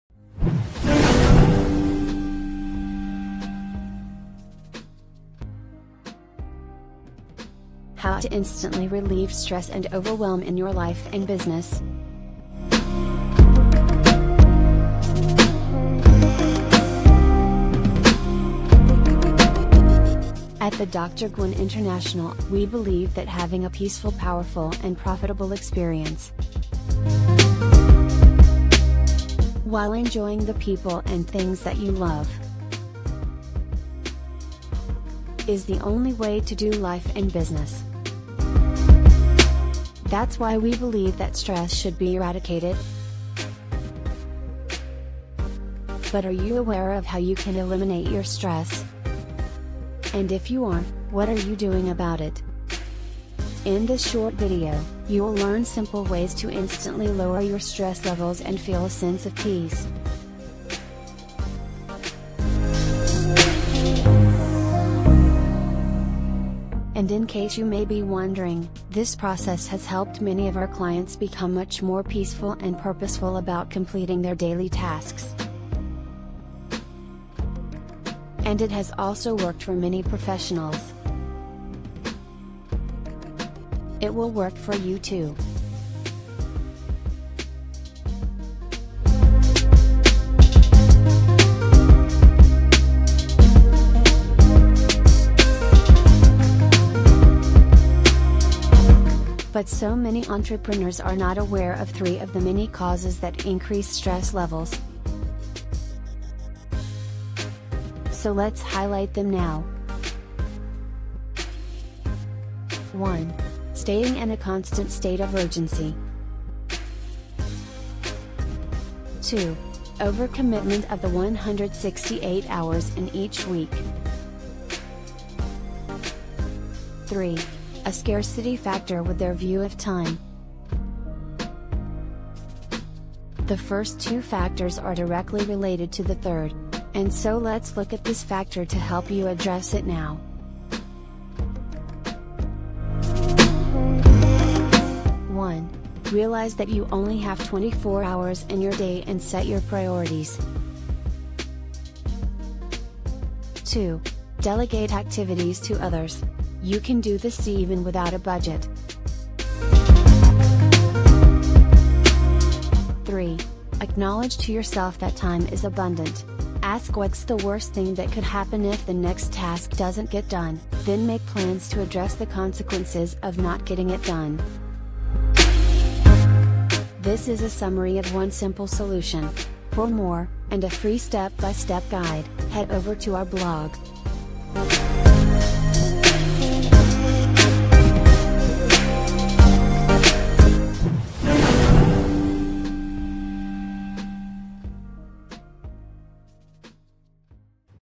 How I Instantly Relieve My Stress
Stress is a huge factor in keeping you from enjoying your experiences and in achieving your goals joyously.

It's a factor that can also reduce your ability to make income. Here is a link to additional details to help you handle stress